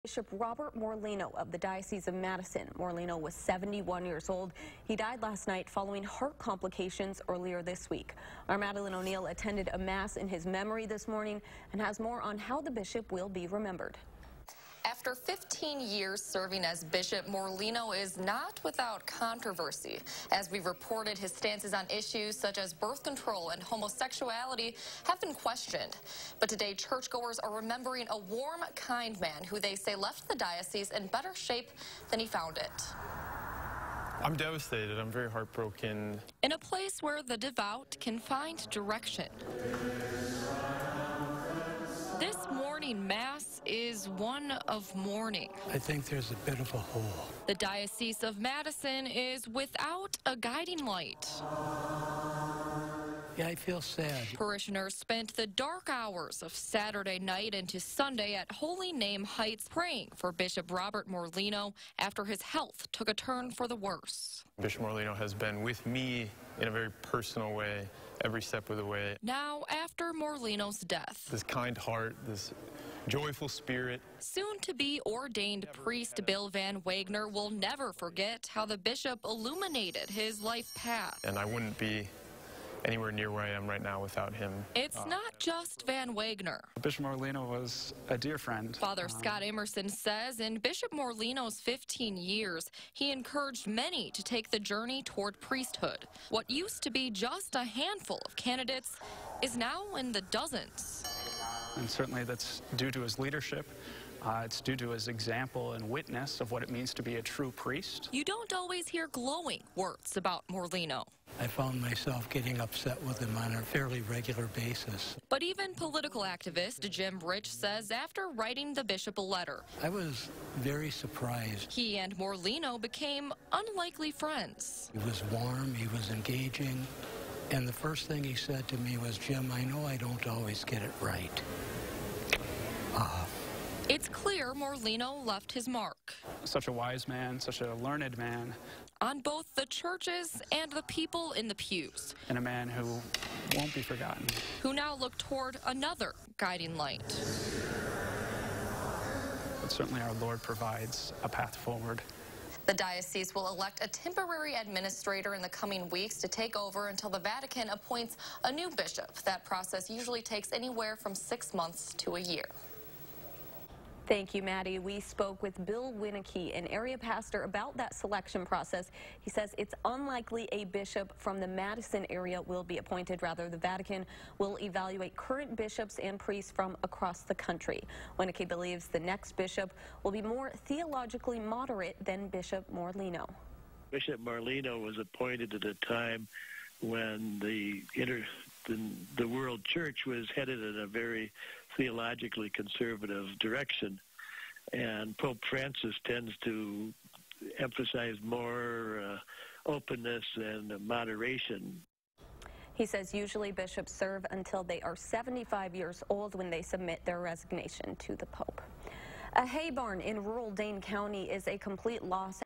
Catholic community mourns death of Bishop Robert Morlino, 'a man who won't be forgotten'
The Catholic community is grieving after the death of Bishop Robert Morlino of the Diocese of Madison, who is dead at age 71 following heart complications during the past week.